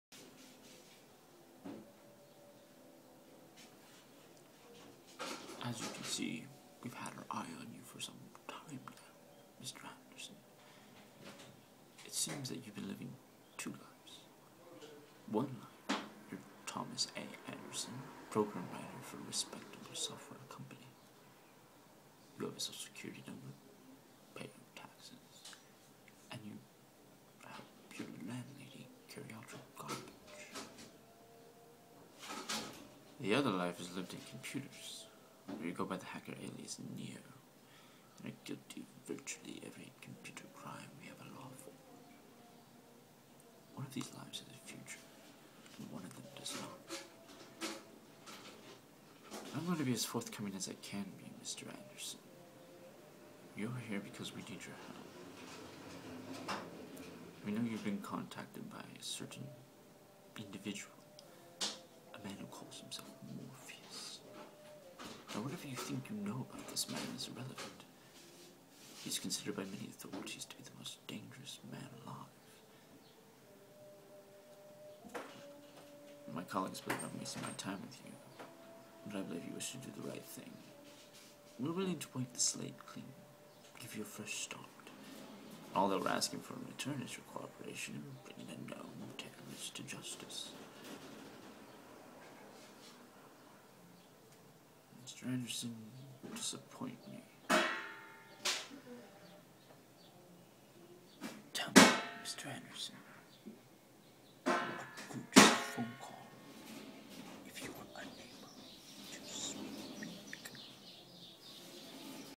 Nathan D. Snyder | Agent Smith | The Matrix | Character Impression | Comedic Digital Series Audition
Character impression audition as Agent Smith from "The Matrix" for a comedic digital series.